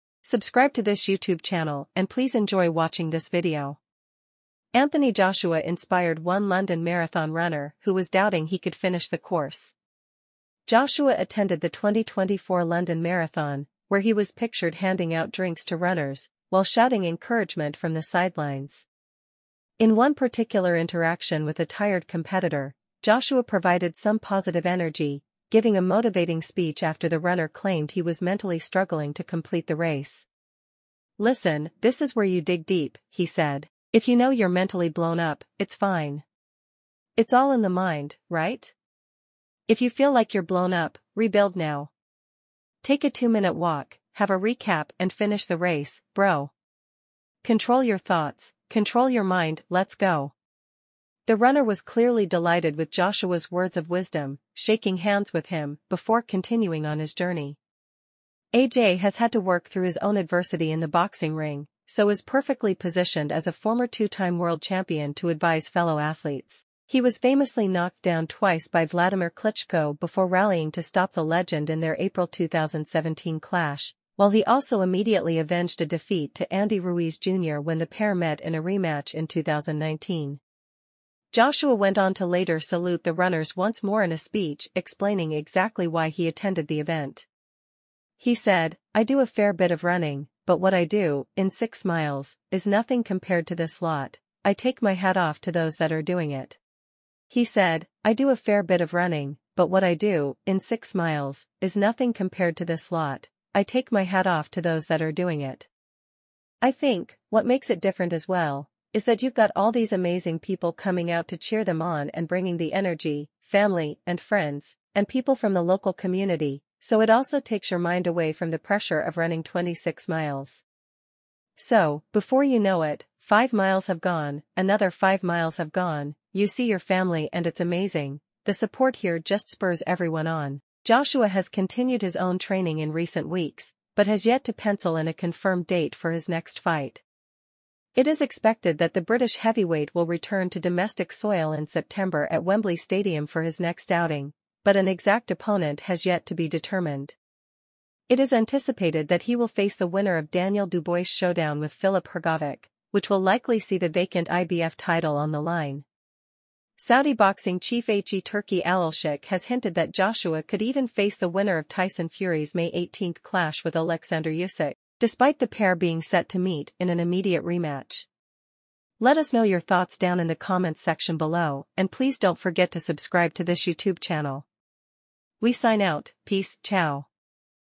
‘What a legend’ – Anthony Joshua gives inspirational pep talk to London Marathon runner during race
Anthony Joshua inspired one London Marathon runner who was doubting he could finish the course.

Joshua attended the 2024 London Marathon, where he was pictured handing out drinks to runners, while shouting encouragement from the sidelines.